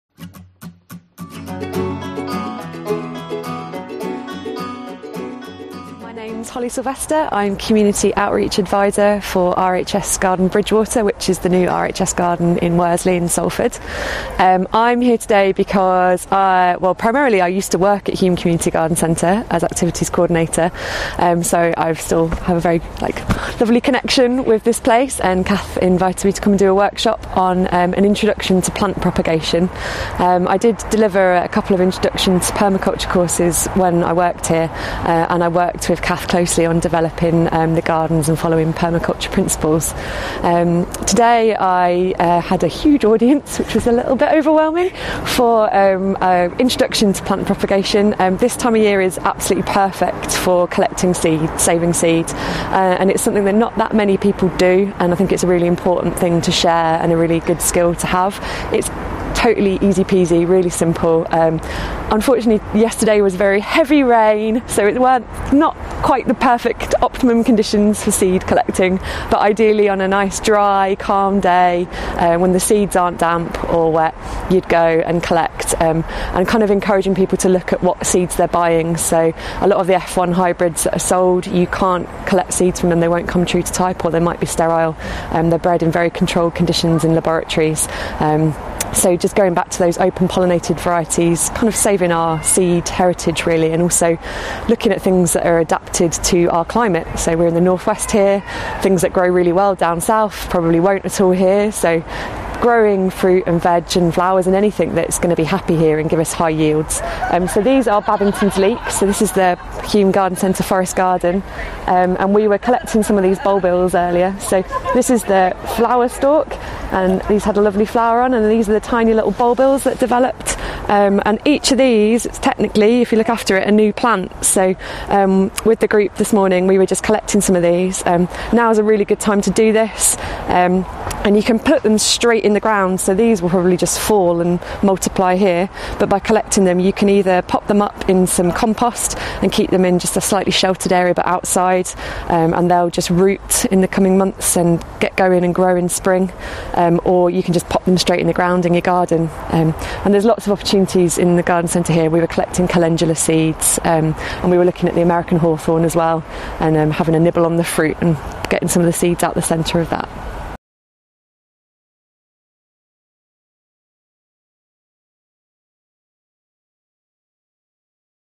Seedsaving | Babington's leek
WeLoveLiving so we save seeds to grow more, locally adapted healthy plants.
Holly Silvester of RHS Bridgewater shows how you can save seed. Holly demonstrates Babingtons leek, a perennial that grows in Hulme Community Garden Centre's forest garden. "It's totally easy peasy, very simple."

Filmed at the national permaculture convergence, September 2018, Hulme Community Garden Centre, Manchester.